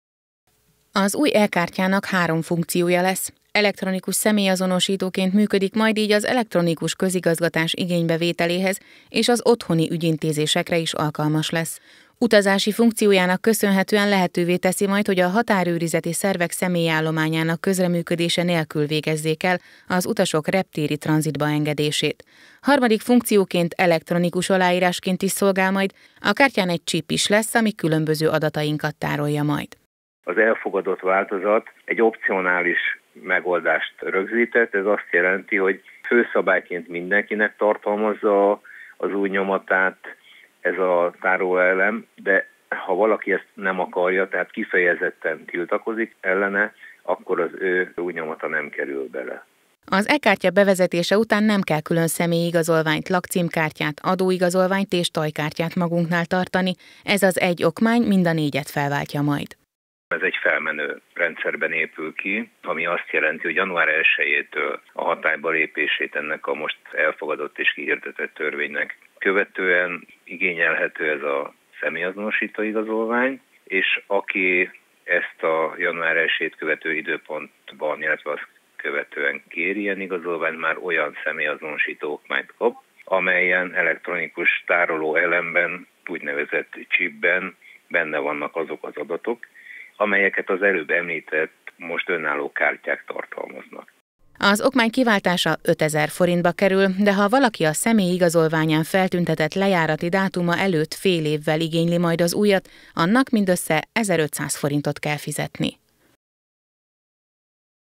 Januártól jön az e-kártya
Januártól kezdve vezeti be a kormány a chip-pel ellátott e-kártyákat. Az új okmány a második generációs biometrikus adatainkat is tárolja, ami azt jelenti, hogy az ujjlenyomatunk is rákerül a tároló chipekre. Az új okmány 5000 forintba kerül majd.